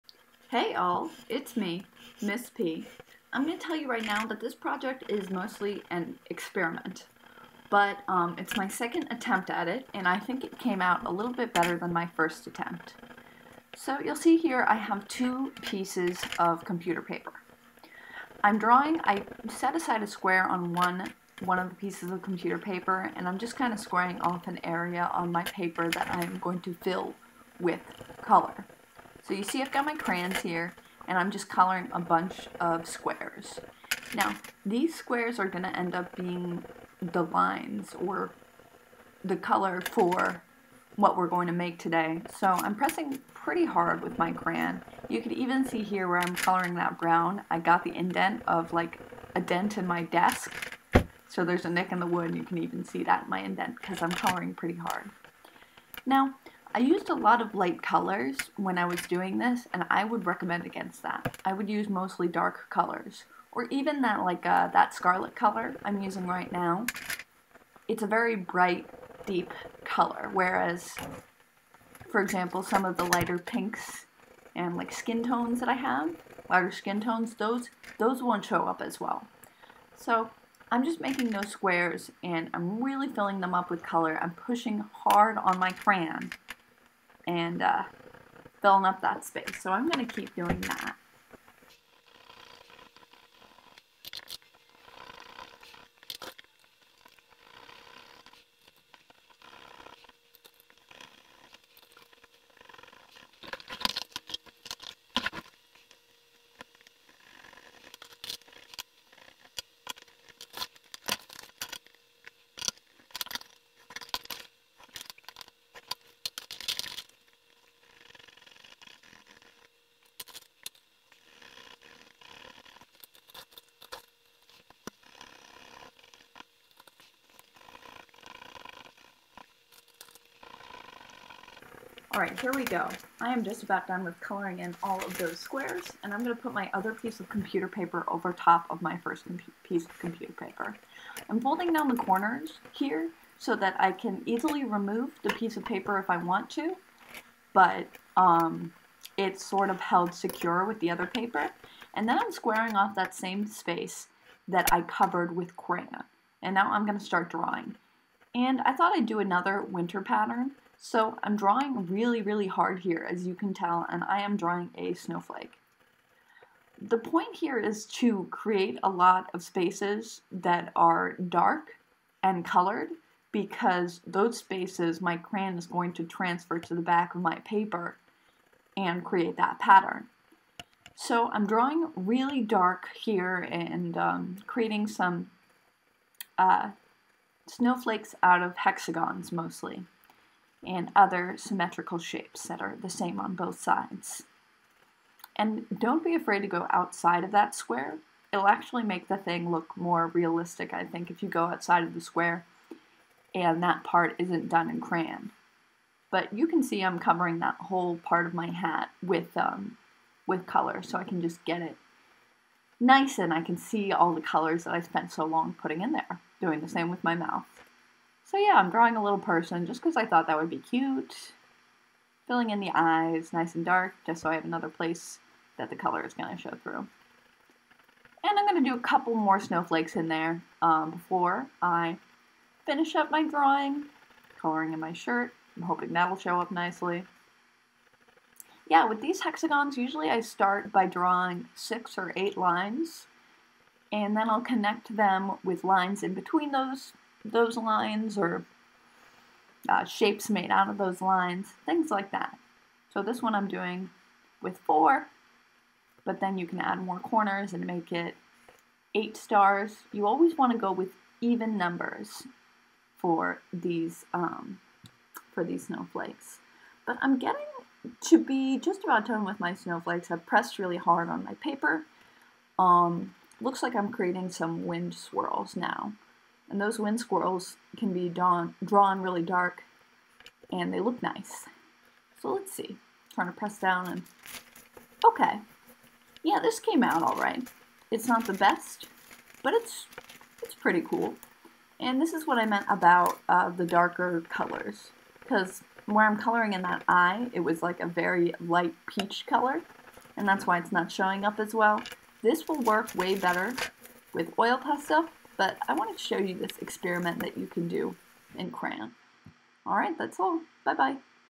Experimenting With Crayon Transfer Prints
Crayon relief can look nice, but it'll always look better with oil pastel. If you don't have oil pastels, you can try with crayon!